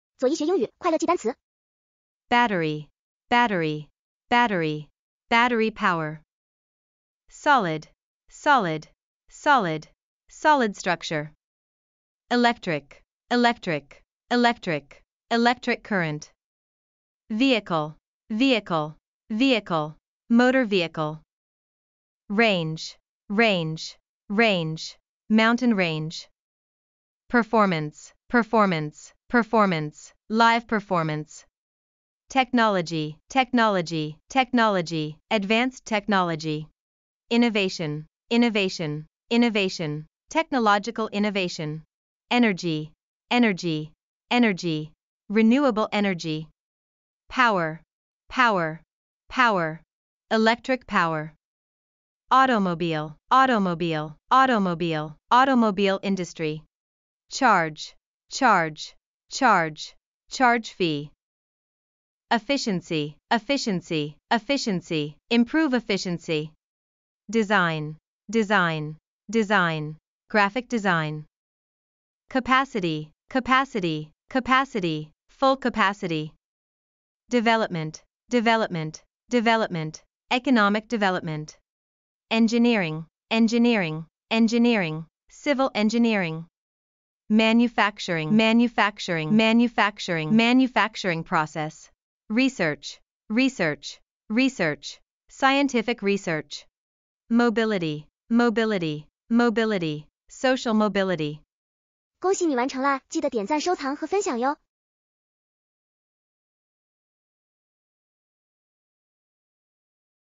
奔驰固态电池续航近千公里♥️热词赏
本视频围绕“奔驰固态电池续航近千公里”主题，介绍 battery（电池）、solid（固态）、electric vehicle（电动汽车）、range（续航）、performance（性能）等核心词汇，讲解先进电池技术及其在汽车设计和能源效率方面的创新，助力学习者理解新能源汽车领域的重要术语和技术发展。